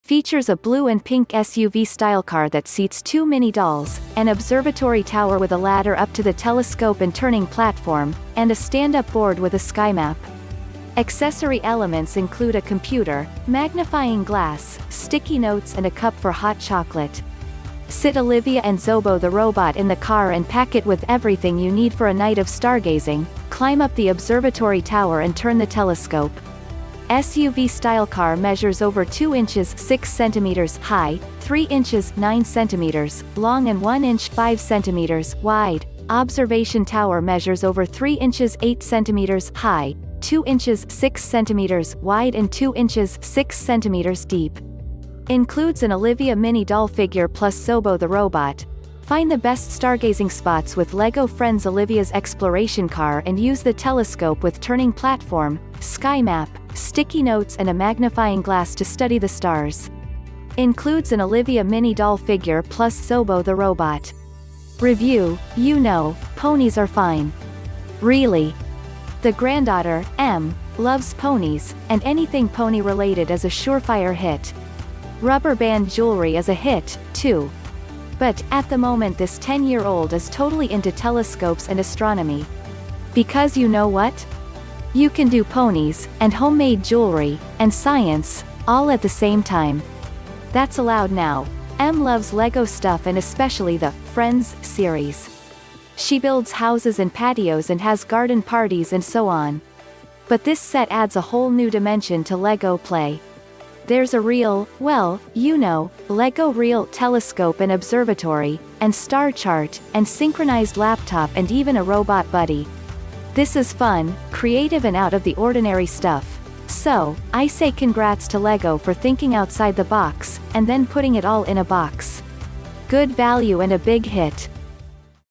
LEGO Friends Olivia's Exploration Car 41116
Features a blue and pink SUV-style car that seats 2 mini-dolls, an observatory tower with a ladder up to the telescope and turning platform, and a stand-up board with a sky map.
Accessory elements include a computer, magnifying glass, sticky notes and a cup for hot chocolate.
Sit Olivia and Zobo the robot in the car and pack it with everything you need for a night of stargazing, climb up the observatory tower and turn the telescope.
SUV-style car measures over 2'' (6cm) high, 3'' (9cm) long and 1'' (5cm) wide, observation tower measures over 3'' (8cm) high, 2'' (6cm) wide and 2'' (6cm) deep.
Includes an Olivia mini-doll figure plus Zobo the robot.Find the best stargazing spots with LEGO Friends Olivia's Exploration Car and use the telescope with turning platform, sky map, sticky notes and a magnifying glass to study the stars. Includes an Olivia mini-doll figure plus Zobo the robot. Review:
  You know, ponies are fine. Really. The granddaughter, M, loves ponies, and anything pony related is a surefire hit. Rubber band jewelry is a hit, too. But, at the moment this 10 year old is totally into telescopes and astronomy. Because you know what? You can do ponies, and homemade jewelry, and science - all at the same time. That's allowed now.M loves LEGO stuff and especially the ''Friends'' series. She builds houses and patios and has garden parties and so on. But this set adds a whole new dimension to LEGO play. There's a real, (well, you know, LEGO-real), telescope and observatory, and star chart, and synchronized laptop and even a robot buddy. This is fun, creative and out-of-the-ordinary stuff.So, I say congrats to LEGO for thinking outside the box, and then putting it all in a box. Good value and a big hit.